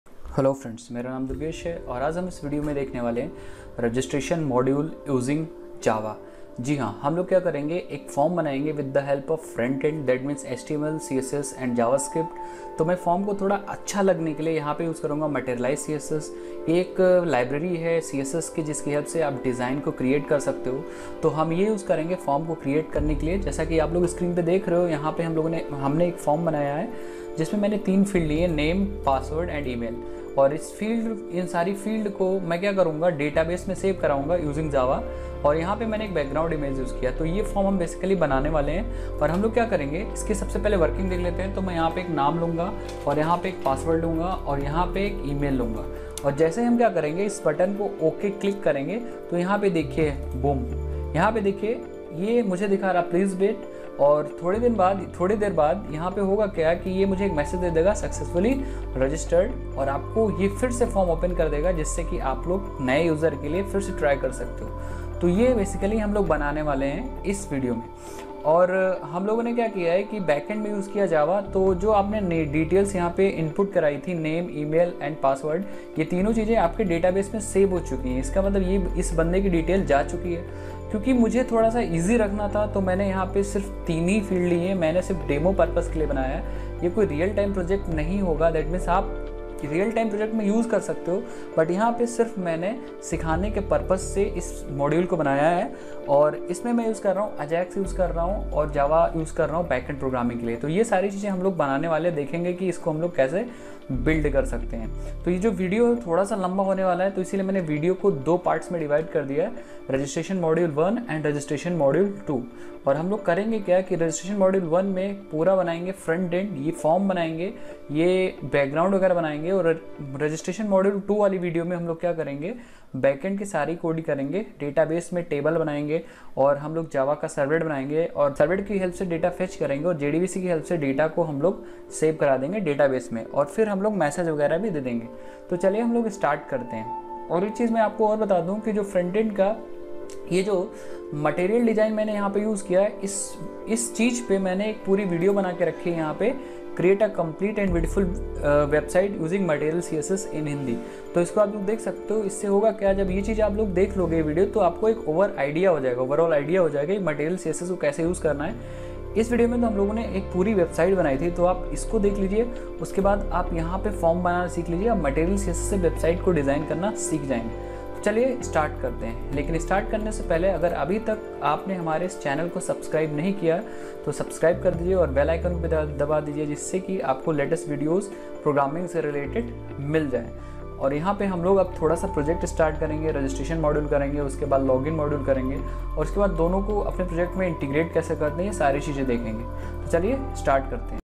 Registration Module #0 | Create Complete Ajax Based Registration Module using Java Servlet in hindi
In this video we will learn how introduction to creating Registration Module using Java Servlet and JSP,